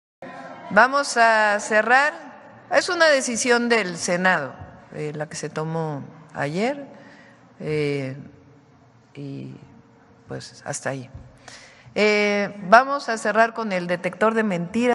Claudia Sheinbaum se pronuncia sobre la reelección de Rosario Piedra en la CNDH
La presidenta Claudia Sheinbaum se pronunció brevemente sobre la reelección de Rosario Piedra al frente de la CNDH, afirmando que la decisión tomada ayer corresponde al Senado.